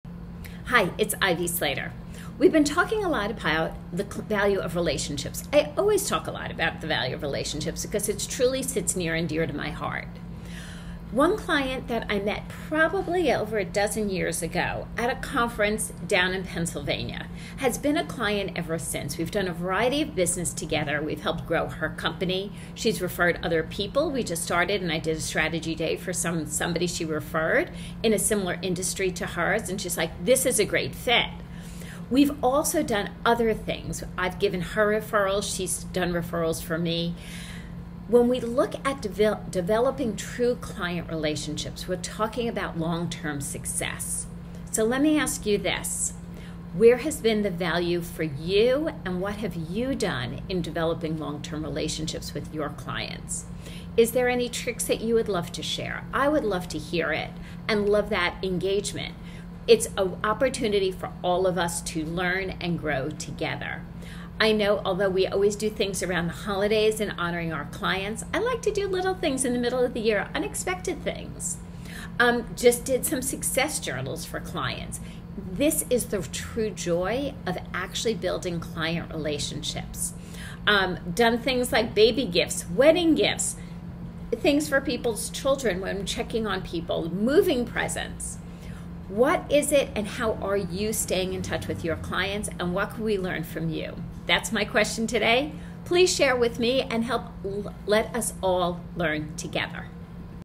How Client Relationships Impact Long-Term Success
The value of relationships truly sits near and dear to my heart.

One client that I met over a dozen years ago at a conference down in Pennsylvania has been a client ever since. We've done a variety of business together.

Slater Success has helped grow her company. She's referred other people.

I just did a strategy day for somebody she referred in a similar industry to hers, and she's like, "This is a great thing".

We've also done other things together. I've given her referrals. She's done referrals for me.

When we look at developing true client relationships, we're talking about long-term success. So let me ask you this.

Where has been the value for you?
What have you done in developing long-term relationships with your clients?
Are there any tricks that you would love to share?

I would love to hear it and love that engagement.
It's an opportunity for all of us to learn and grow together.

I know, although we always do things around the holidays to honor our clients, I like to do little things in the middle of the year, unexpected things. I just did some success journals for clients.

This is the true joy of actually building client relationships. I've done things like baby gifts, wedding gifts, things for people's children.

How are you staying in touch with your clients, and what can we learn from you?

Please share with me and help us all learn together.